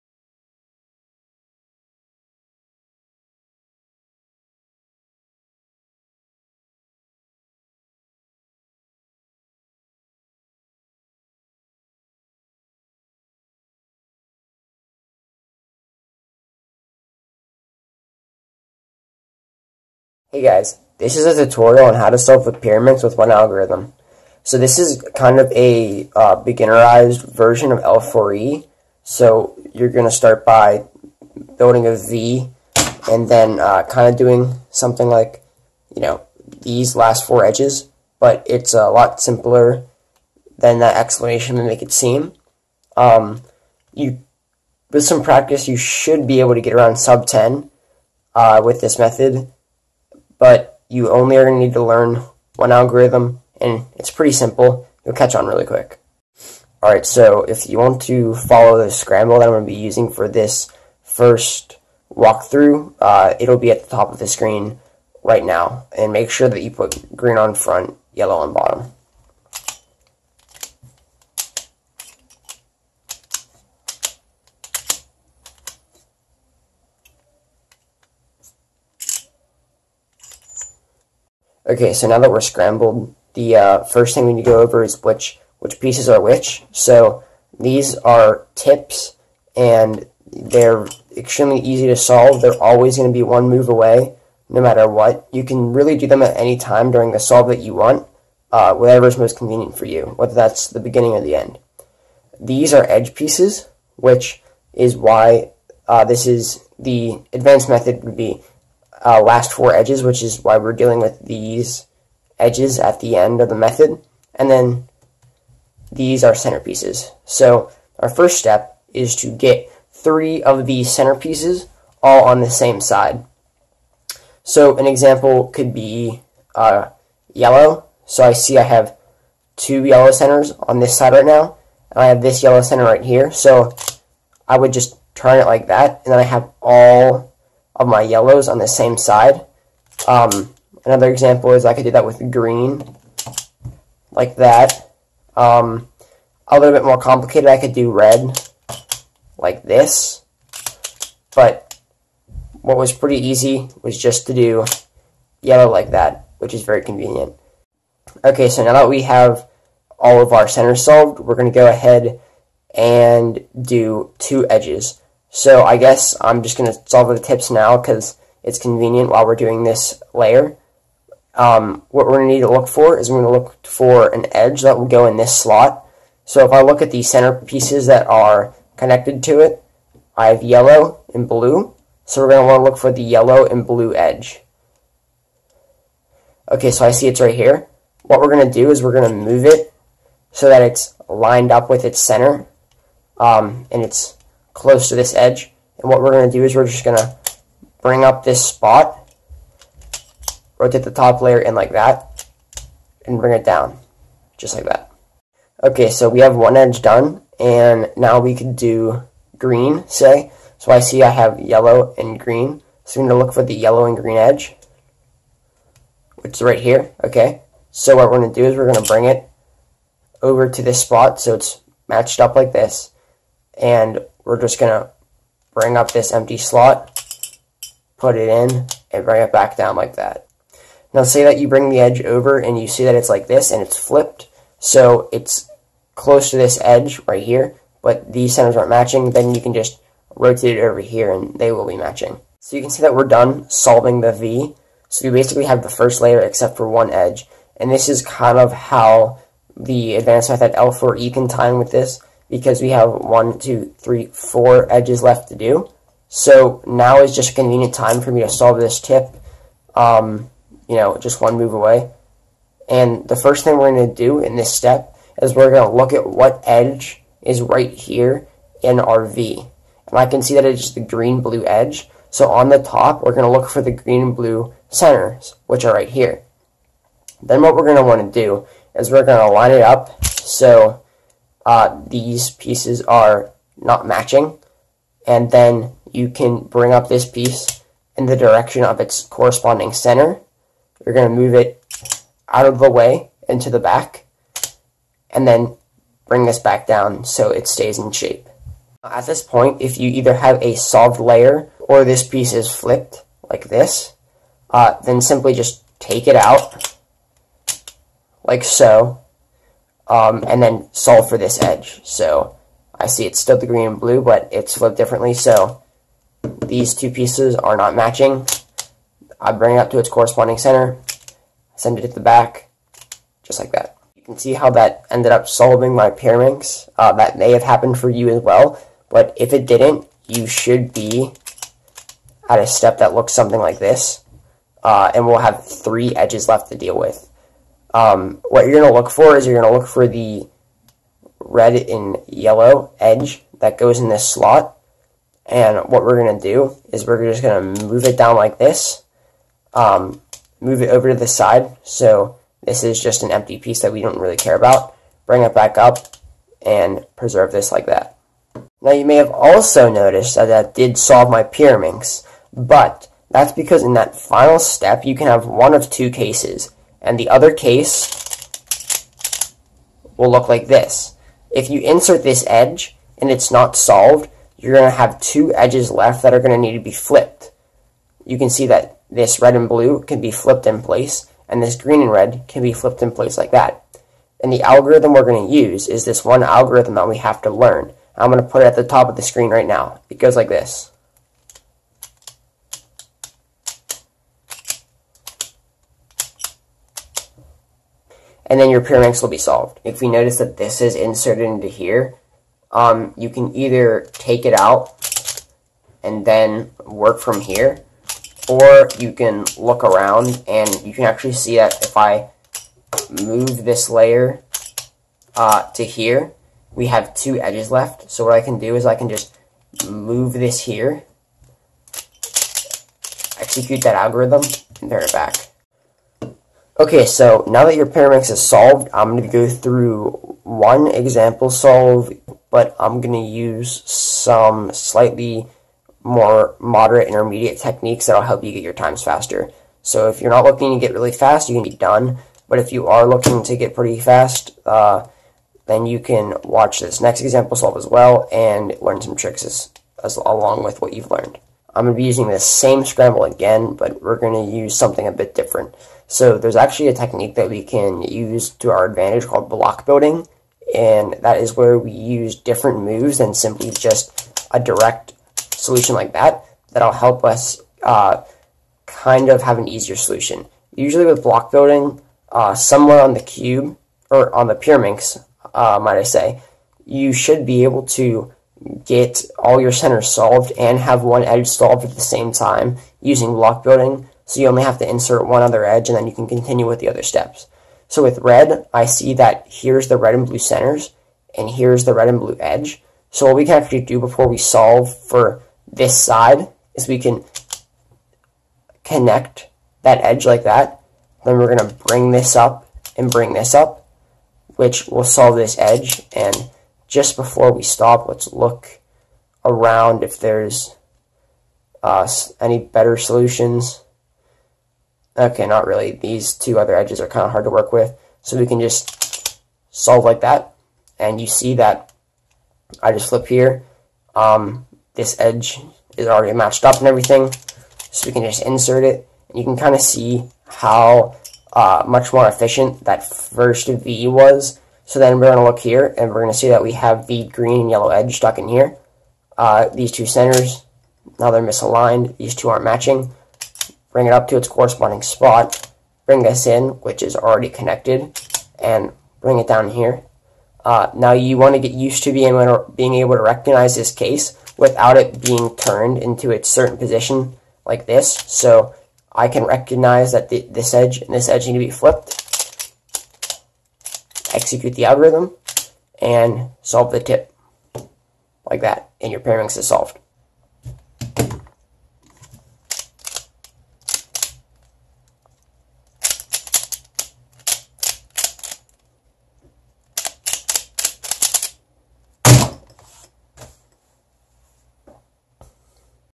How to Solve a Pyraminx with one Algorithm | Beginner L4E
This method may not be as fast as layer by layer, but it gets the job done!

Scramble: U B' L' U' R L U R' l r u'
Corner flip alg: R' L R L' U L' U' L

The 86 is pronounced with my name, it is not just a number.